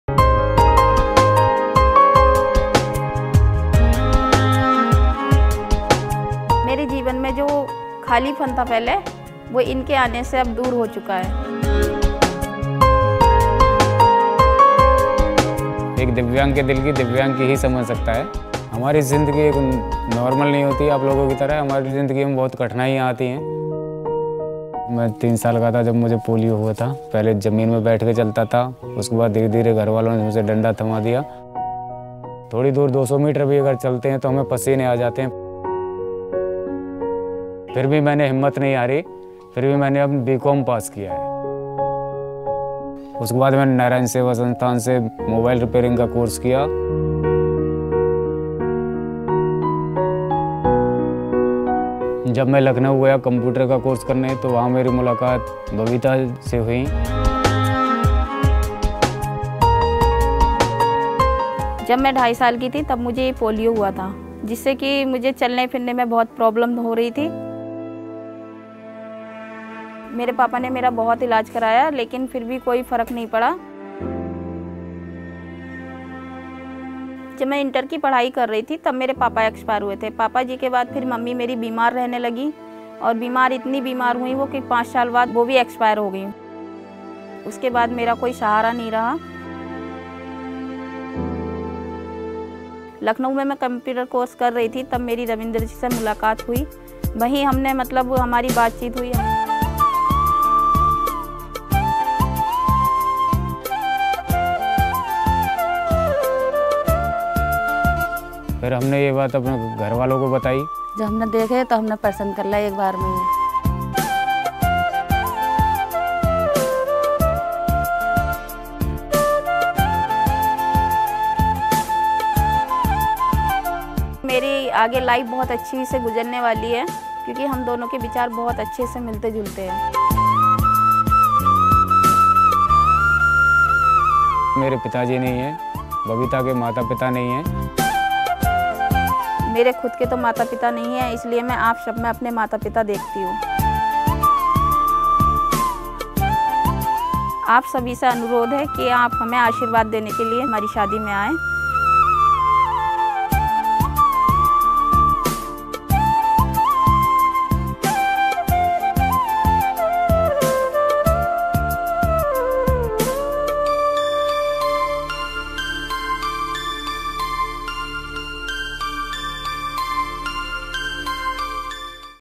विवाह बंधन में बंध कर साथ होंगे सकलांग | Divyang Vivah 2022 | नारायण सेवा संस्थान
रविंद्र और बबिता, अपने जीवन के नए सफर को नारायण सेवा संस्थान में होने जा रहे दिव्यांग विवाह से शुरू करने जा रहे हैं।
इनका अब तक का जीवन कैसा रहा और कैसे इन दोनों ने विवाह का यह निर्णय लिया आइये जानते हैं इन्हीं की ज़ुबानी।
इन्हें आशीर्वाद देने आप सब अवश्य पधारें।